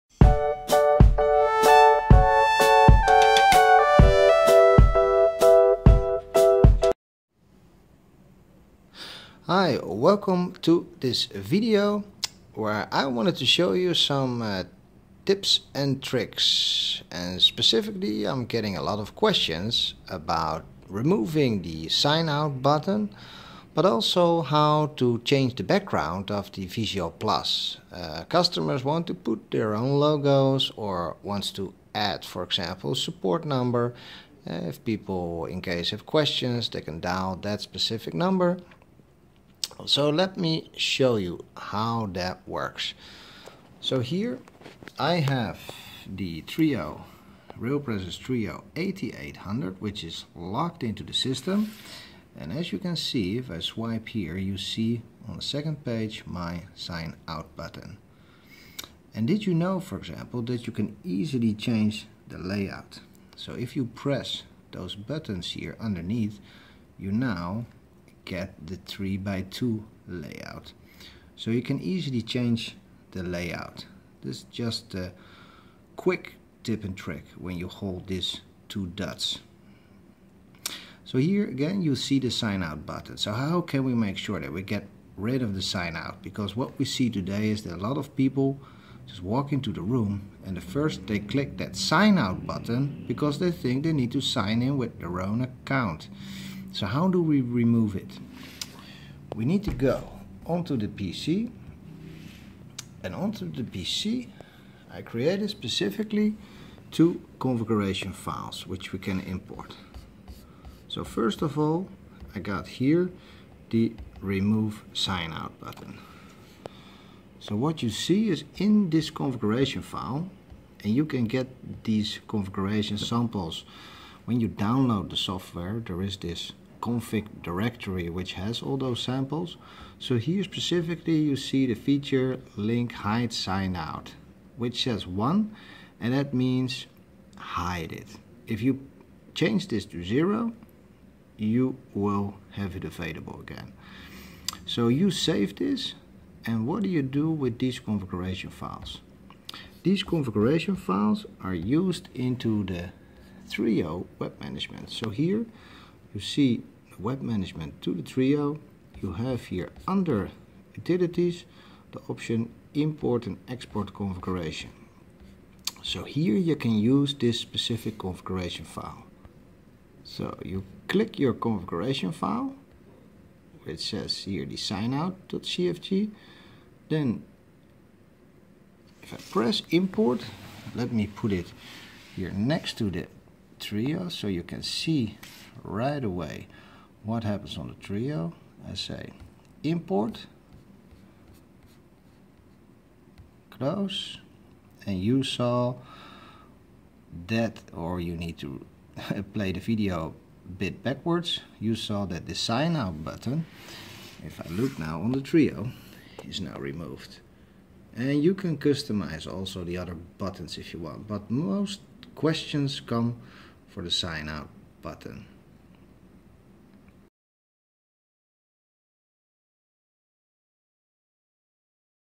Tech Tip: How to remove Polycom Trio Sign-out button
This video shows how easy it is to remove sign-out button ( or any other button) on the Trio. See here link to sample config files